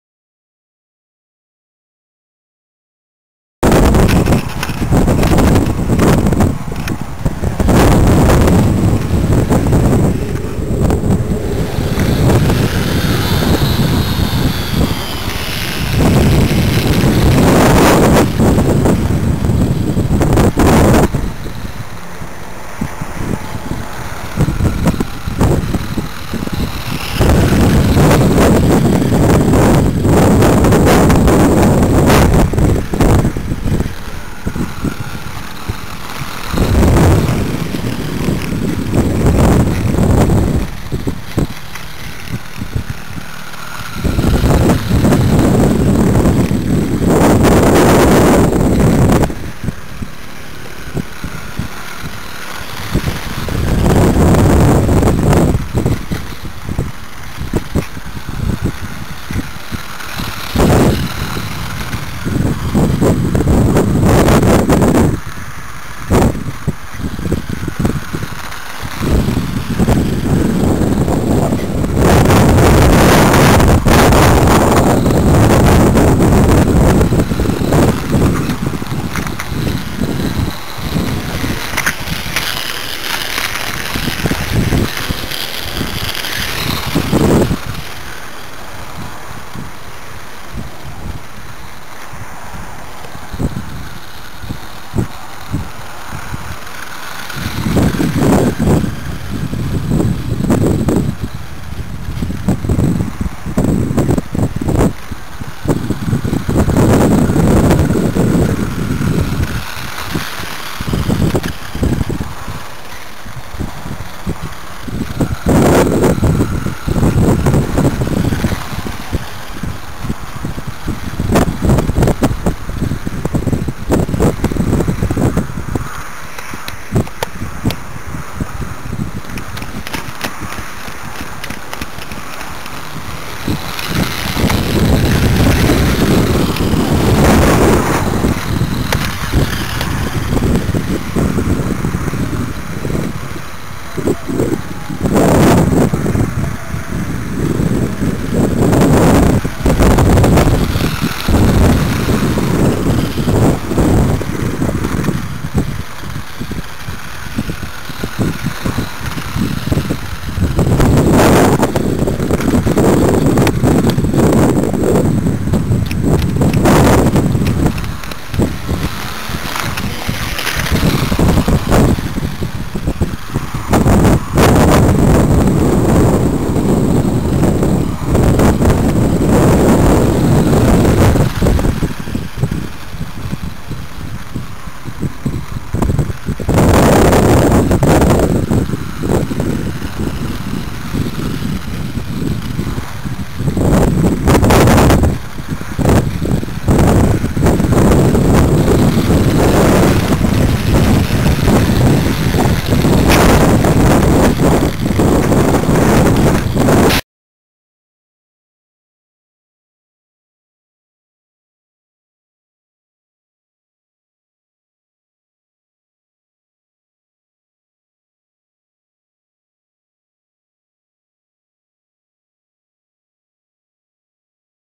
両さん自転車R/C
1/6スケールの、自転車型R/Cです。内蔵のコマジャイロにより、安定性を高めています。両さん人形がペダルをこぐ姿は、非常にユーモラスです。